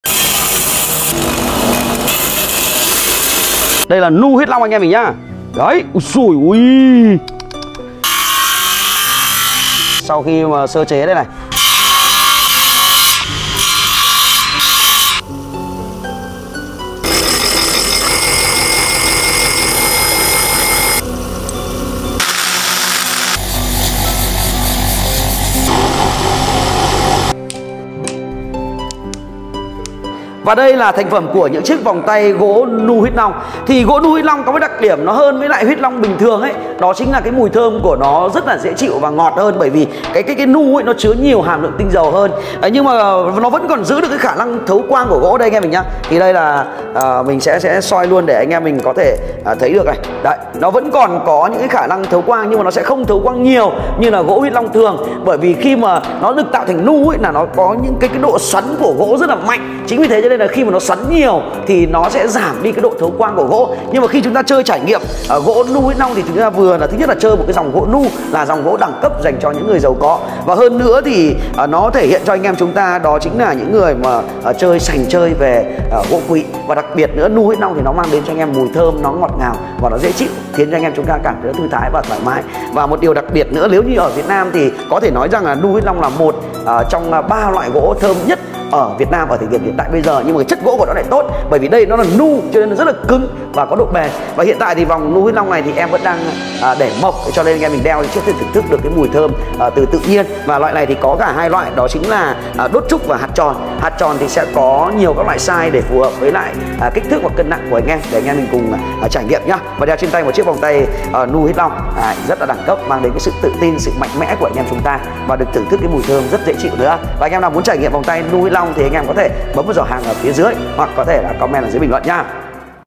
TIẾT LỘ Quá Trình Chế Tác Ra Vòng Tay | Bật Mí Bí Quyết Kinh Doanh | Đồ Gỗ Mạnh Sơn
Bật mí quá trình chế tạo ra những chiếc vòng tay trên thị trường hiện nay. Bí quyết kinh doanh mà không cơ sở sản xuất nào tiết lộ.

🚩Địa Chỉ: 125A Làng Nghề Thiết Úng - Vân Hà - Đông Anh - Hà Nội